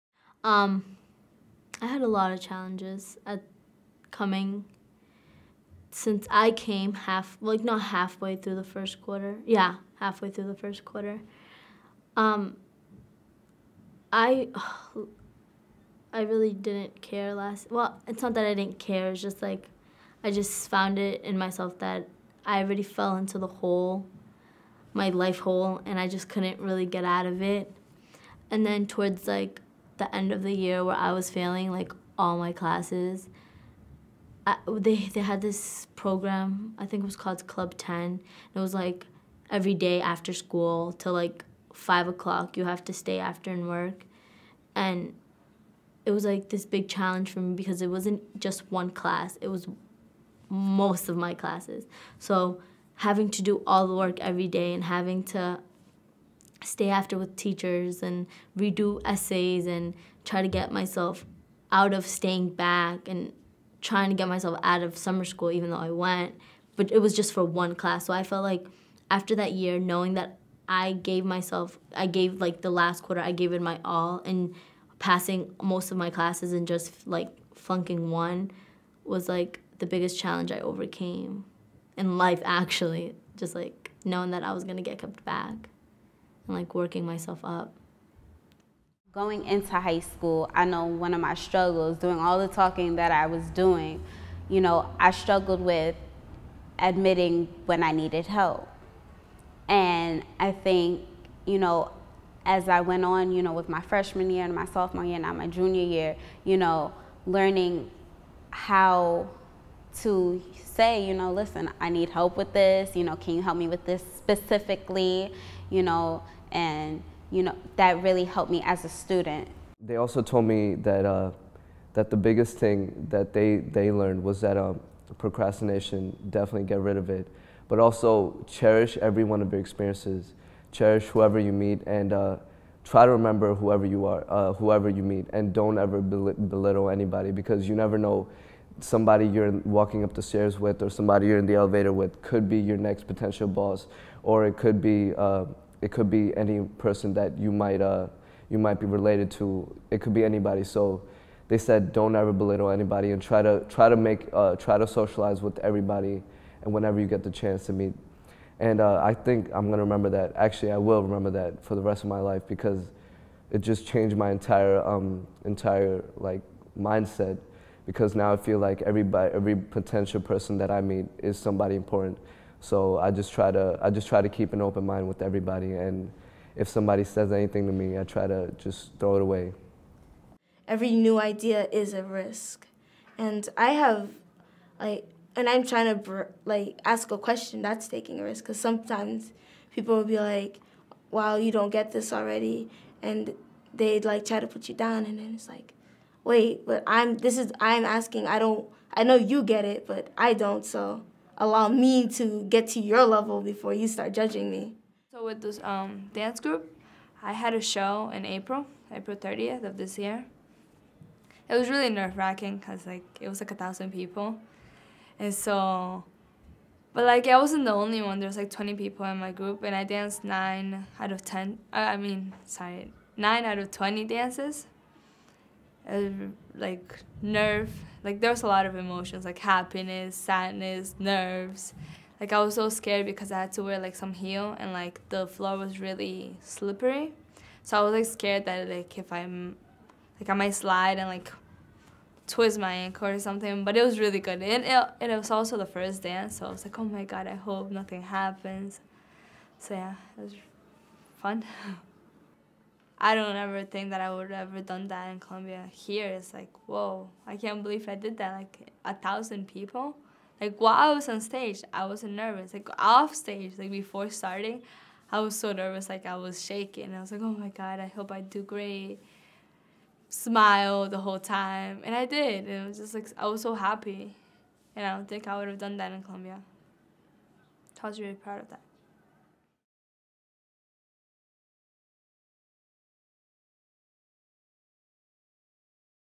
Rising to the Challenge
In this collection of short video clips, part of WKCD's Just Listen series, students talk about  rising to the challenges of doing well in school and looking towards college.